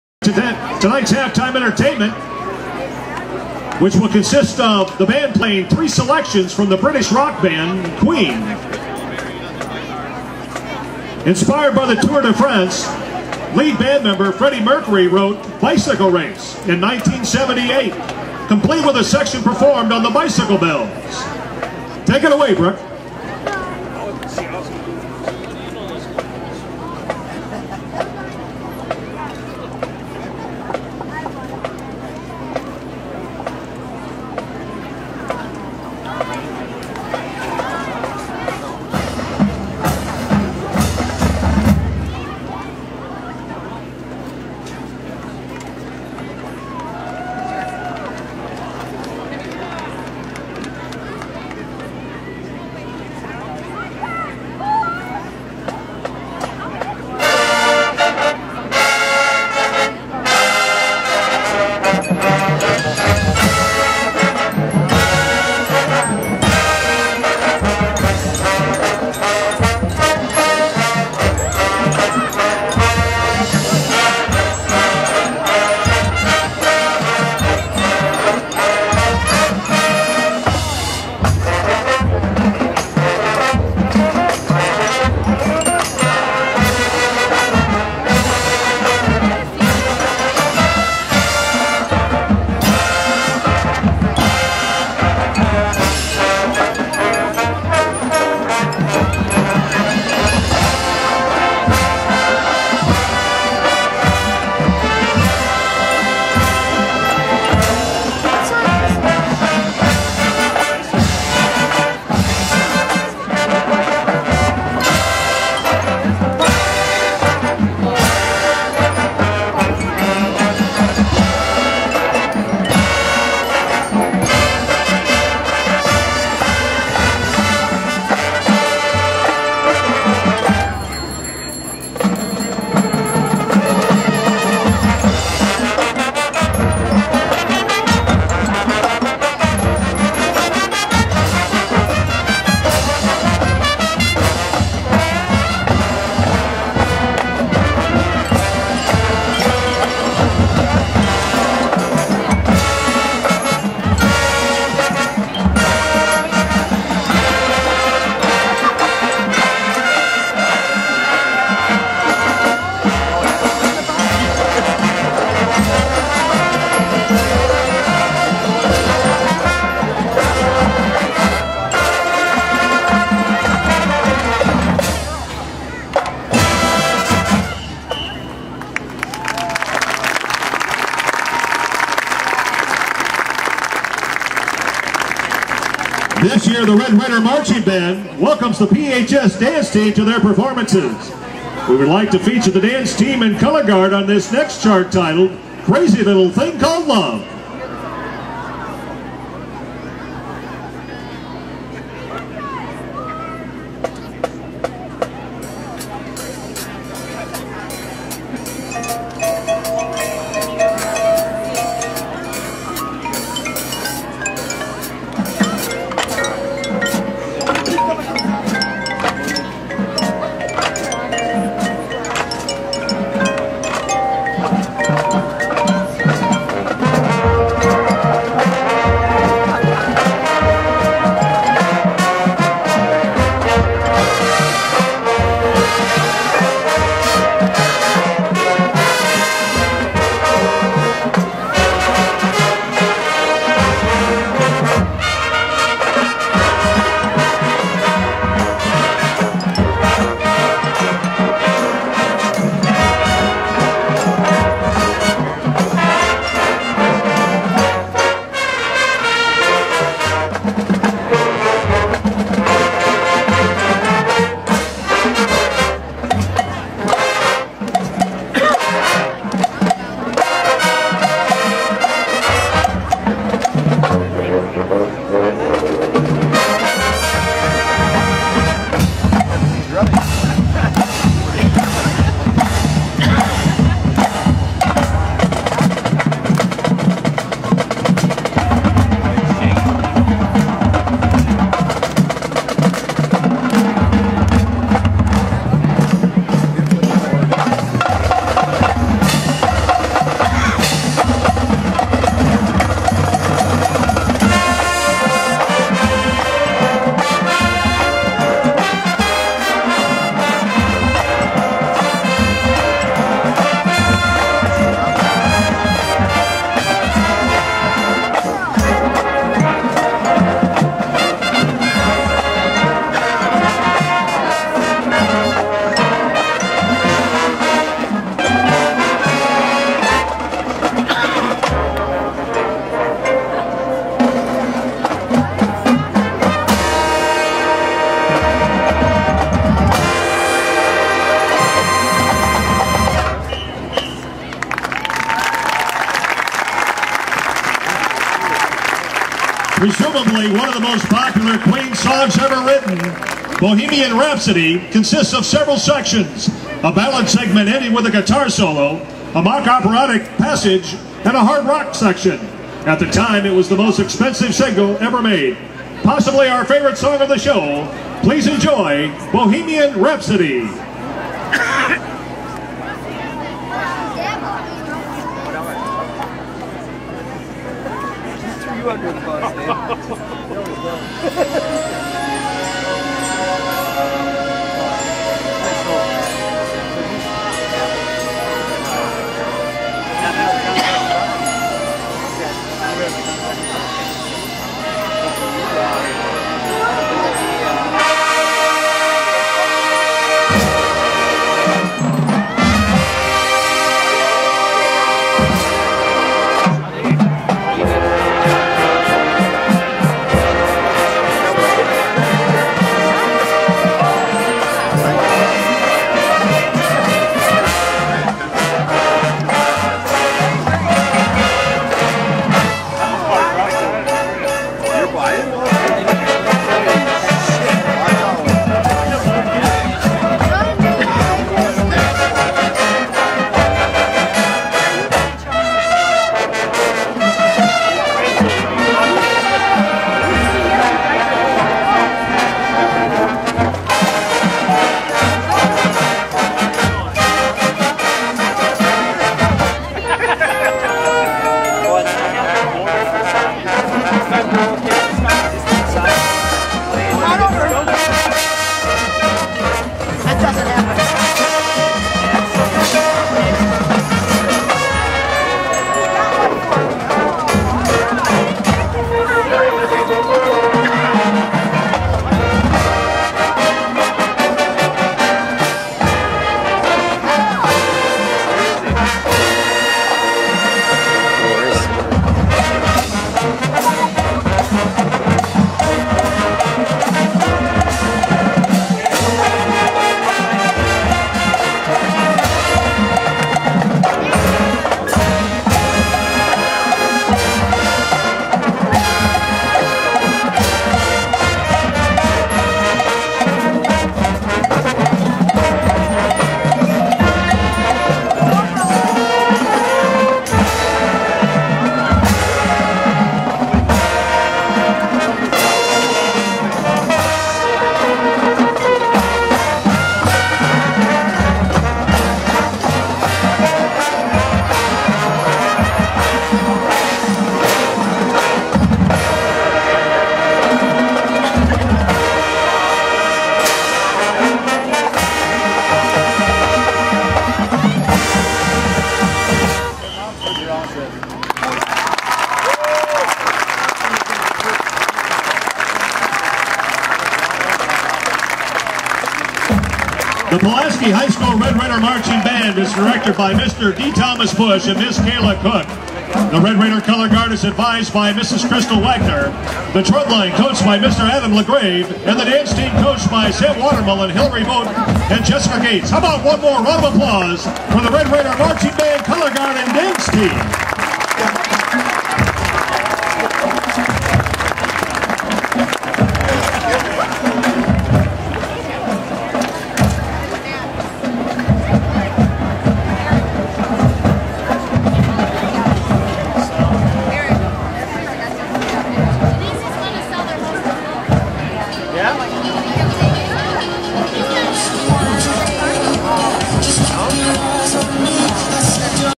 Pulaski Community High School Marching Band Field Show Fall 2015
Pulaski High School's 2015 field show, a medley of Queen tunes.